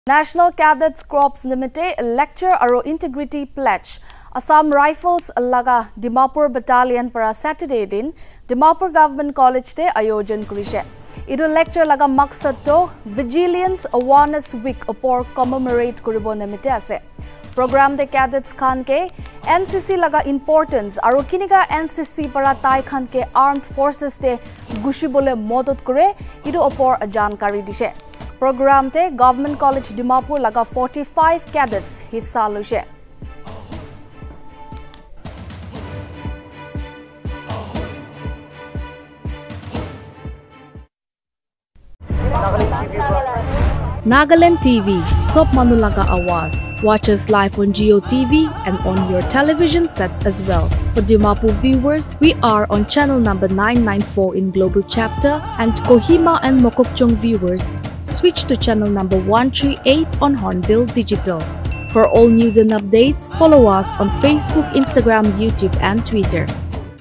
ASSAM RIFLES DIMAPUR BATTALION ORGANIZES LECTURE CUM INTEGRITY PLEDGE
A lecture and integrity pledge for National Cadets Corps was organized by Dimapur Battalion of Assam Rifles under the aegis of Inspector General of Assam Rifles (North) at Dimapur Government College on November 5. Notably, the aim of the lecture was to commemorate the vigilance awareness week on the theme "Corruption free India for developed nation". The cadets during the programme were briefed on the importance of NCC and how it can help them in joining armed forces while they were also motivated to promote transparency and honesty amongst them